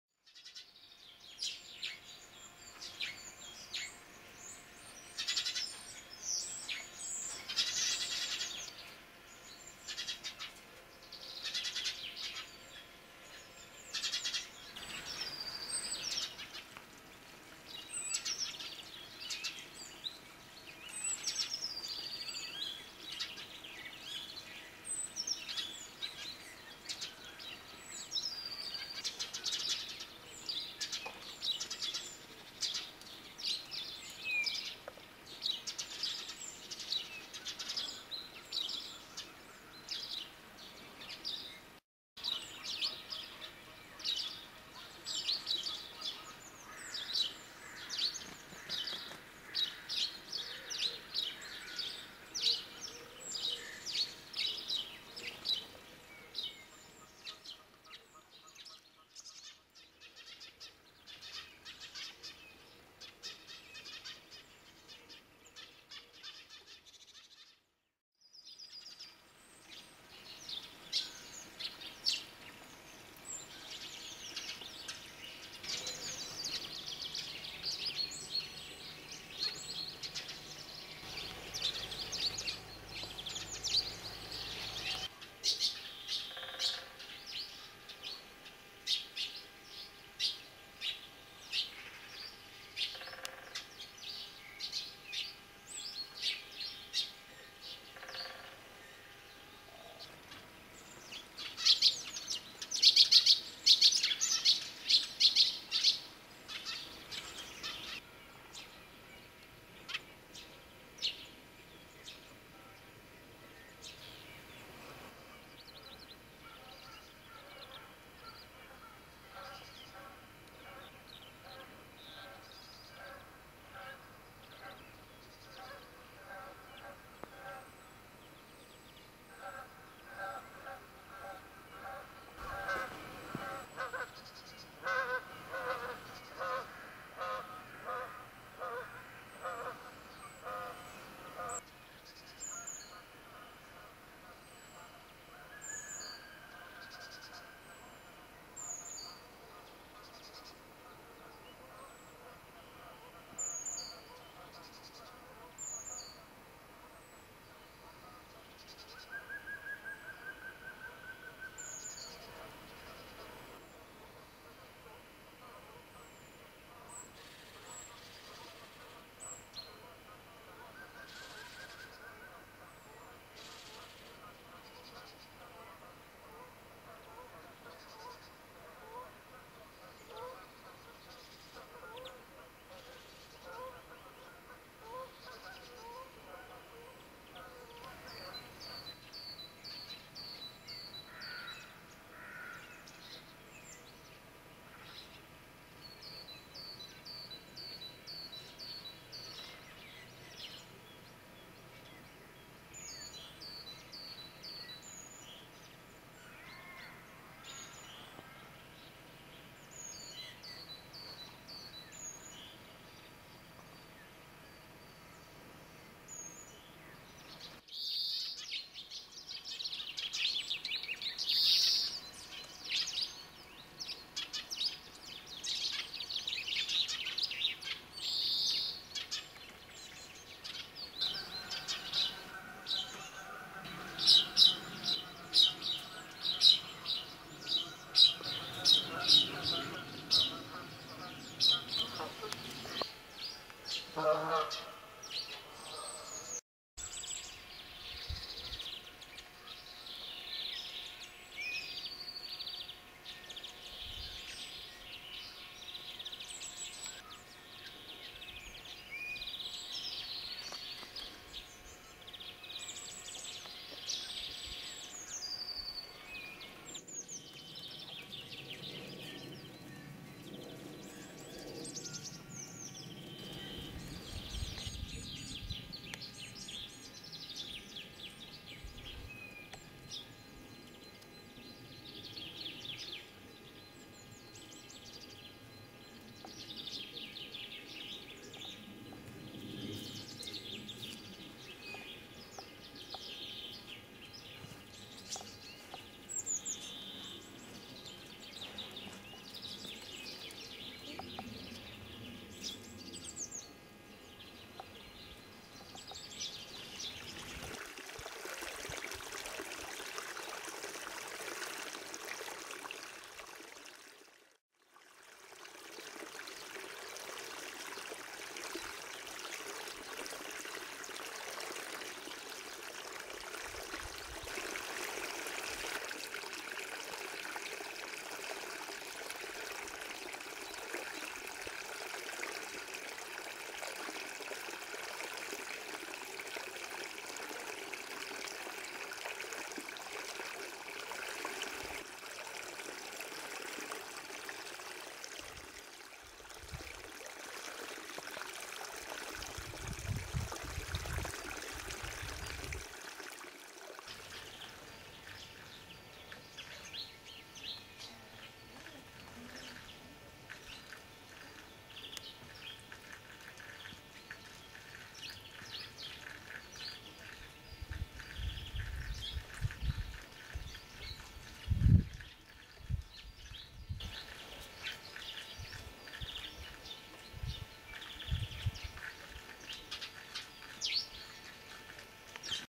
Vårdagjämning 2011.wmv
En fin vårvintermorgon. Kan inte låta bli att pröva filmandets alla möjligheter. En glad amatör har mycket kvar att lära, men övning ger färdighet. Höjd volym rekommenderas.